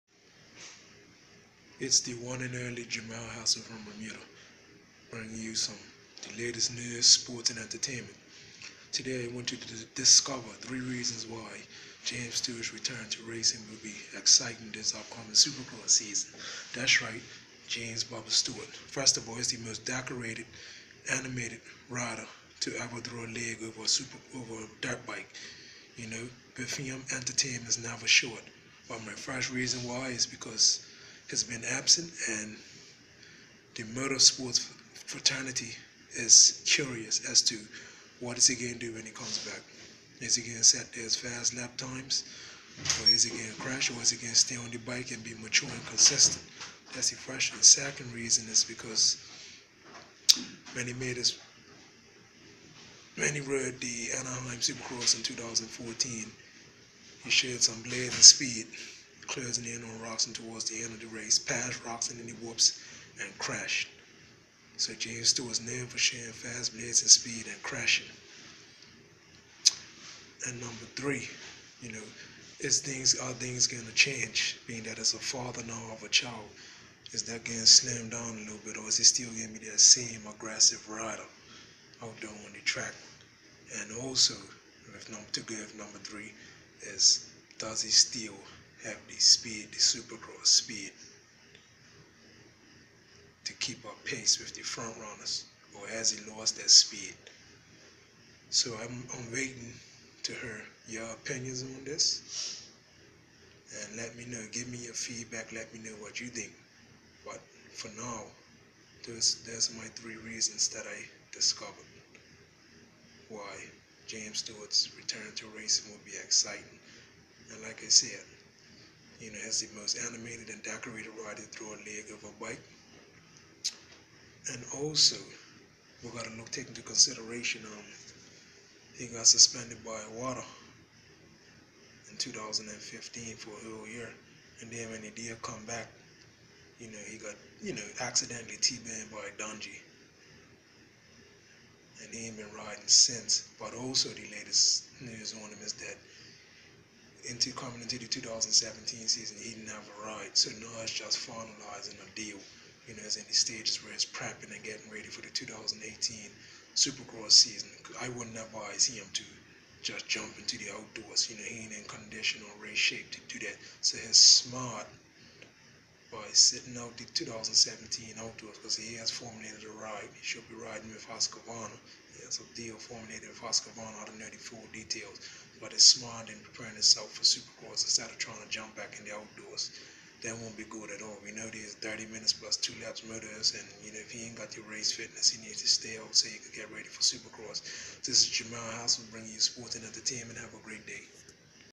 Discover the 3 reasons why James stewarts return to racing will be exciting this upcoming supercross
James bubba Stewart dubbed as the fastest man on the planet makes his long awaited return to racing this upcoming sure cross season. Fans like myself are anxiously waiting for James  return because he is an entertainer and blazing fast  when he is ready.

Join the community and contribute with your comments